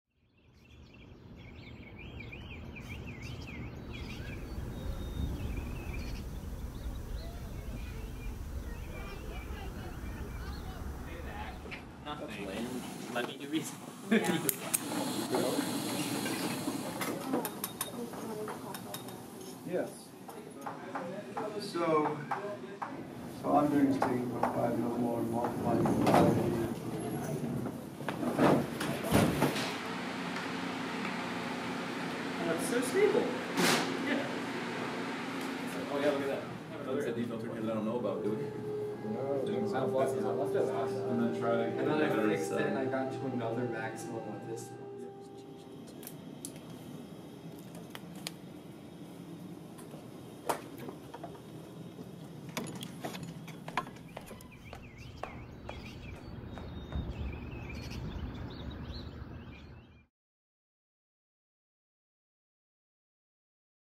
One Minute in the Science Quad
Welcome to the S'Quad, where the buildings are physically connected, because we are connected: to the professors who guide us, to the research that drives us, and to one another (ask us about bologna loaf). Tap the play button, and don't forget your safety glasses.